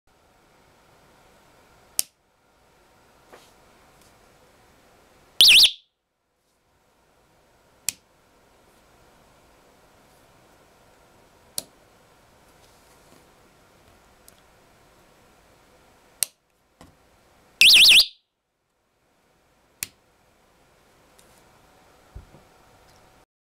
Home Made 2-Way Micro Switch Door Alarm
This is a noise maker I made.  It is basically a 107 db siren attached to a Miniature snap-action switch extended with a paperclip that can push flush with the wood in the front to avoid damaging the switch.  The other switch allows it to change the direction that activates it.  Perfect for pranks and it works for on any kind of hinged opening.